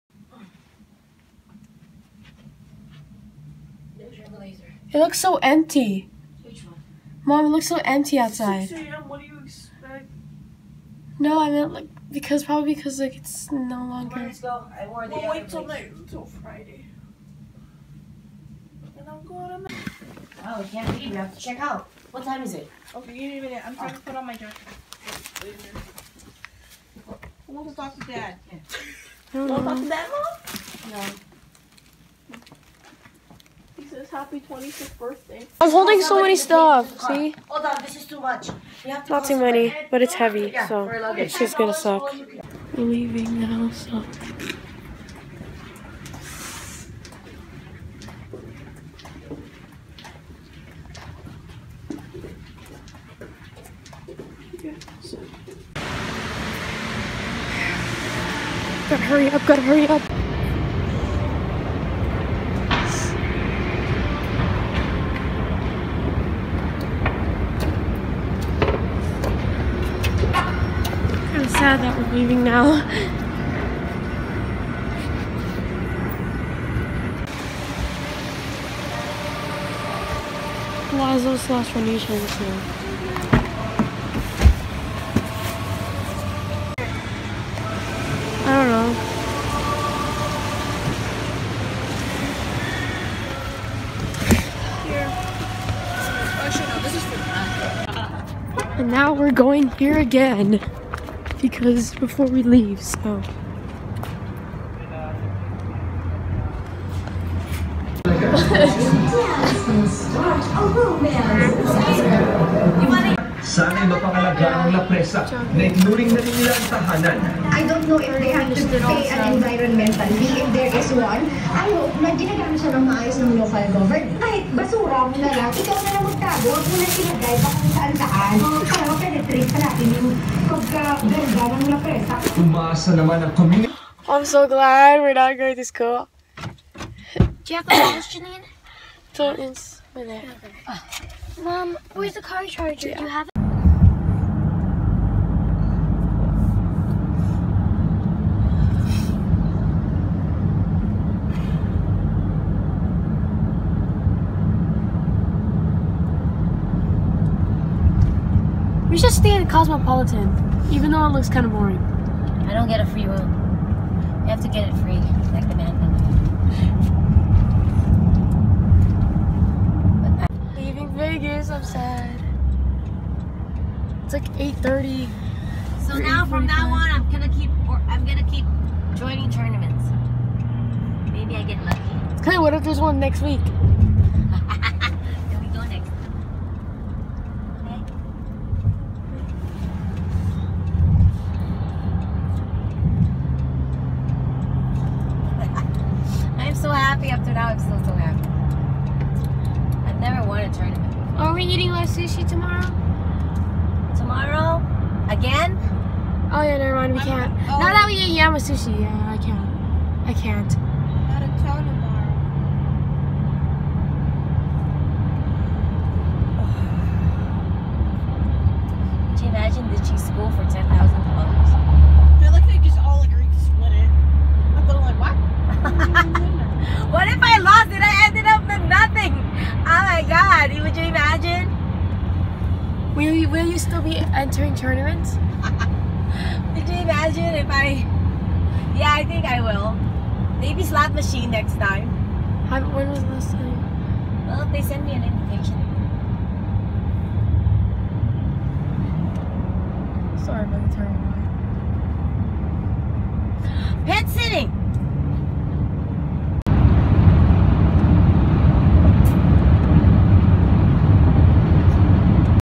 Las Vegas Vlog - Day 4/Final Day (May 11, 2015)
READ ME!
This is not really necessary to watch. This is just a vid of us leaving Vegas.